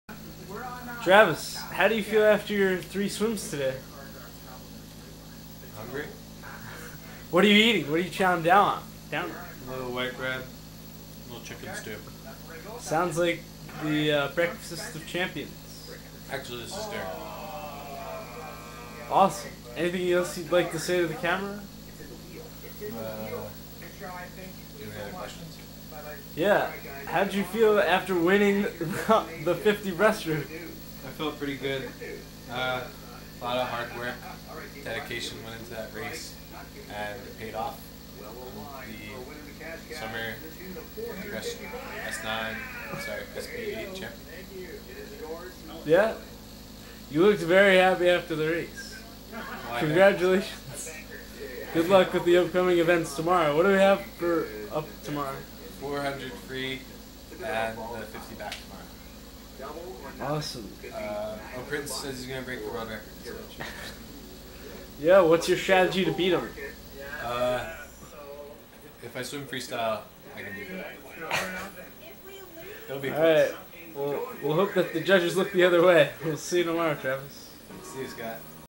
Post Meet Interview 2
Travis gives some insight to his fans after a hard day of swimming. And lets us know what's on the menu! 7/16/2011